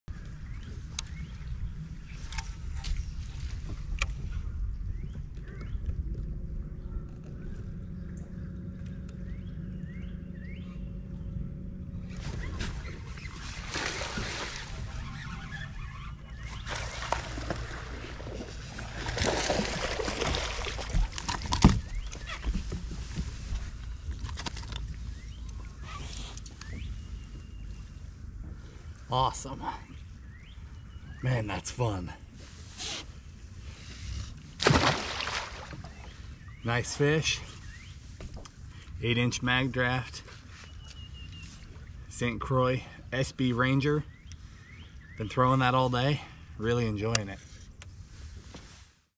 Giants Lurk In The Shadows!
This bass CRUSHED the 8" MagDraft swimbait! Its amazing how these big bass hunker back in the shadows to ambush anything that comes too close. Next time you're fishing around docks, don't be afraid to throw that swimbait back in the shadows.

THE BAIT...
Color: White Back Shad

THE COMBO...